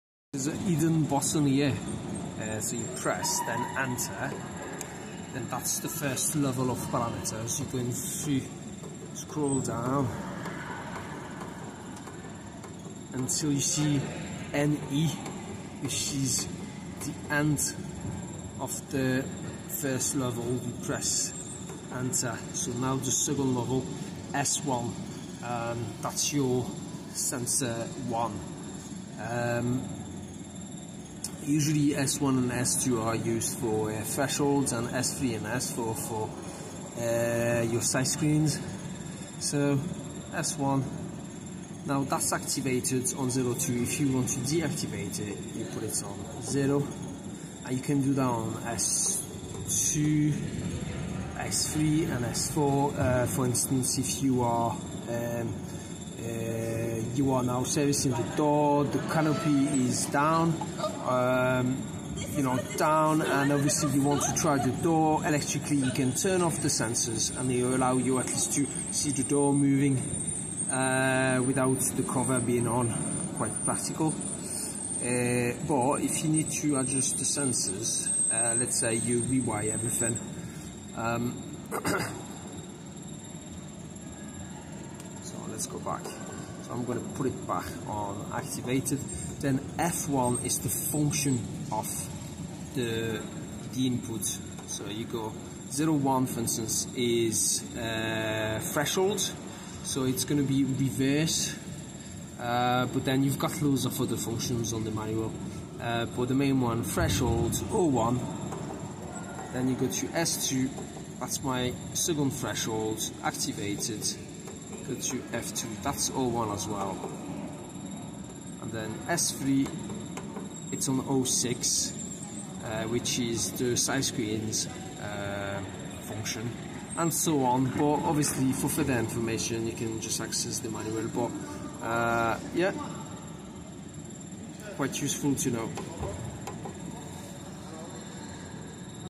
How to turn off sensors and set their functions on GEZE SL Slimdrive
S1, S2, S3 and S4 safety inputs:
-00 Disabled,
-02 Activated
F1, F2, F3 and F4 functions:
-01 SIS reverse
-02 SIS and KI
-03 SIS and KA
-04 SIS slow
-05 SIO stop
-06 SIO slow